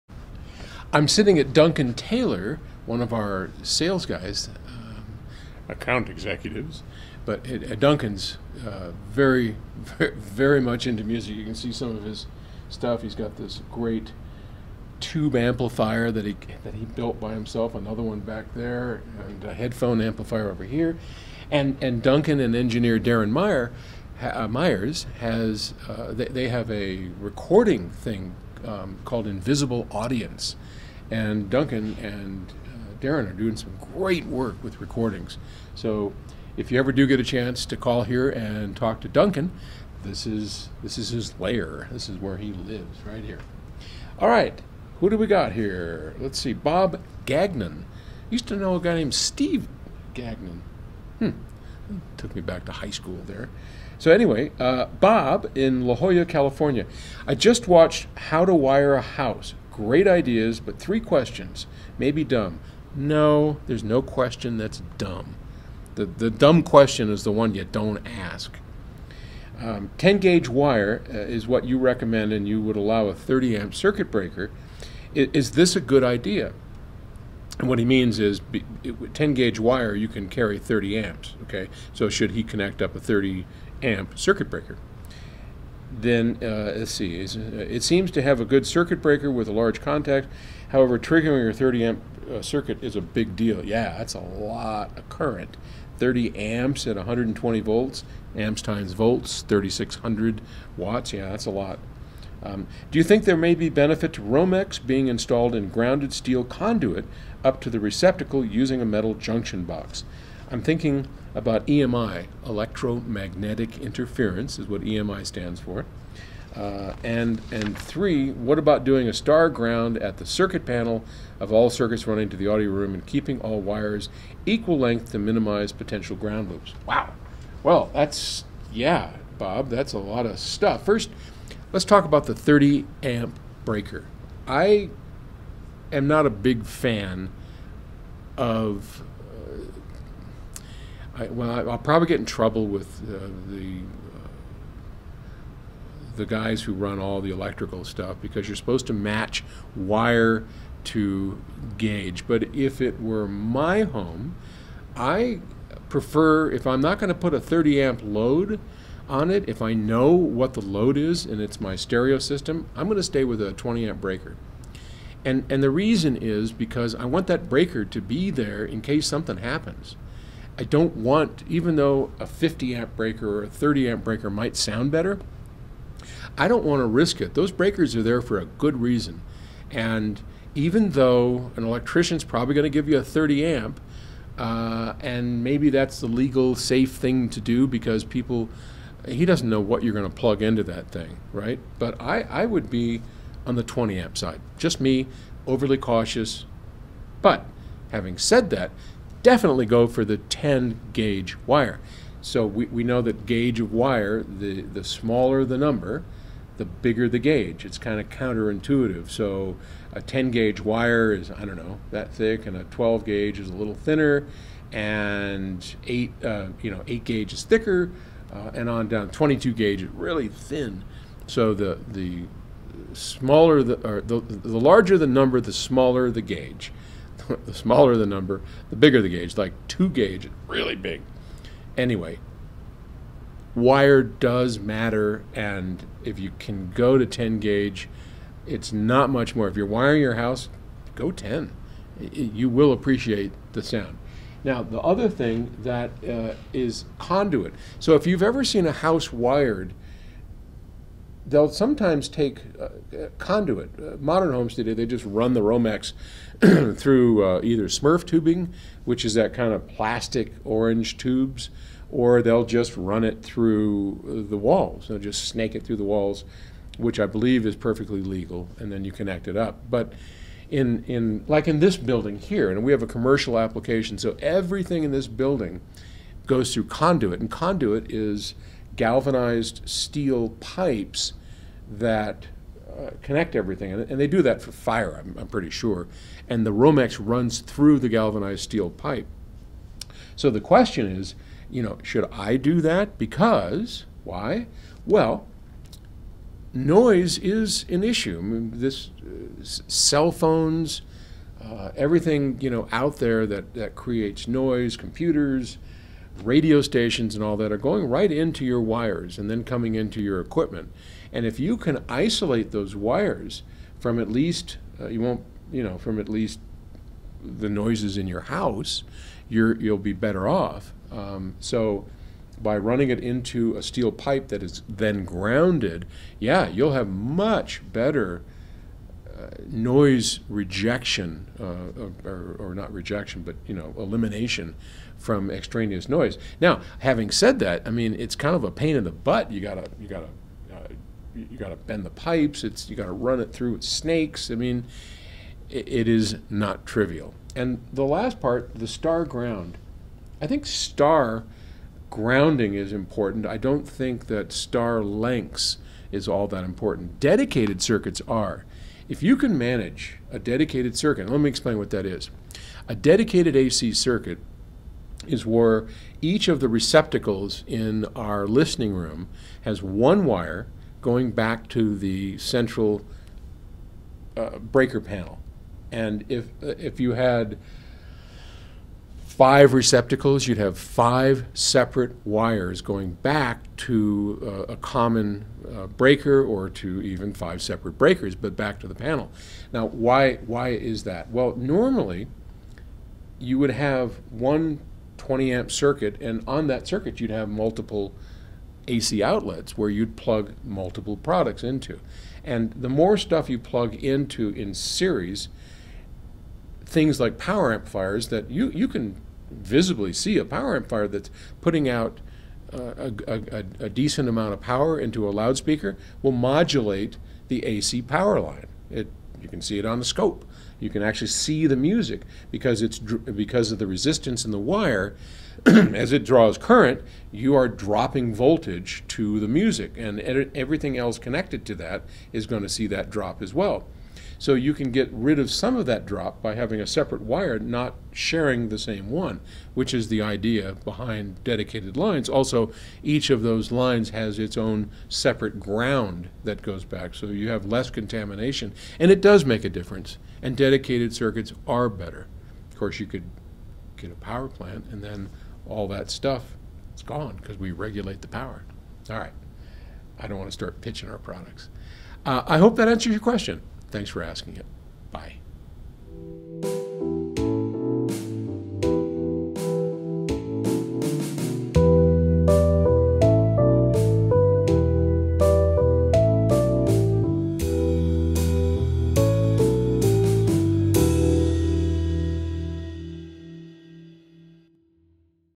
House wiring for high end audio
Does house wiring matter for sound quality and if so, is it a good idea to use grounded metal conduit? What about dedicated lines for audio quality? Do they matter? Circuit breakers? Have a question you'd like Paul to answer?